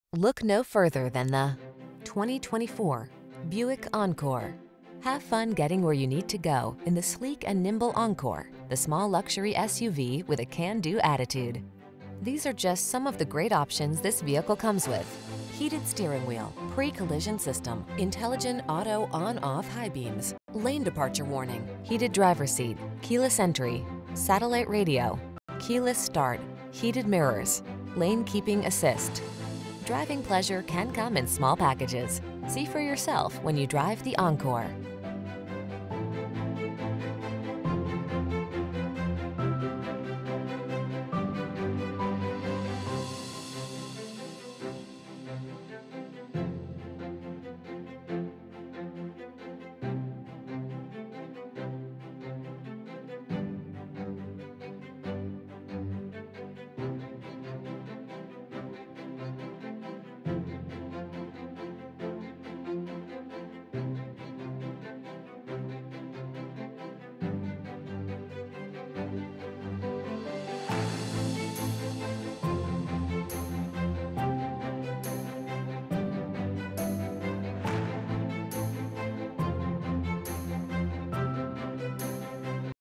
2024 Buick Encore_GX West Plains MO, Mt Home AR, Willow Springs MO, Nixa MO, Mountain Grove MO 24618
Summit White New 2024 Buick Encore_GX Preferred available in West Plains Missouri at Taylor Grubaugh Chevrolet Buick GMC. Servicing the Mt Home AR, Willow Springs MO, Nixa MO, Mountain Grove MO area.